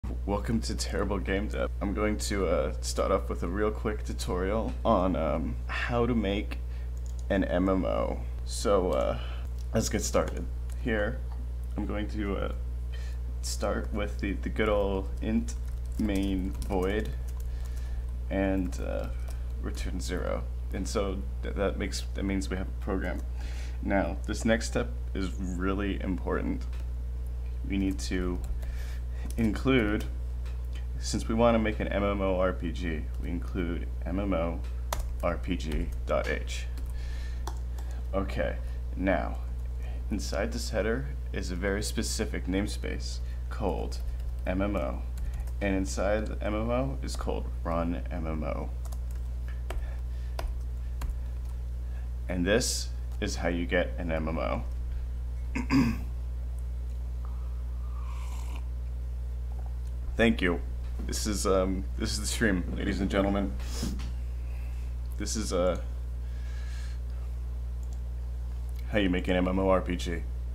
[tutorial] how to make an mmorpg
from stream this morning. my nose is stuffy. this code is available from my github thanks have a pleasant afternoon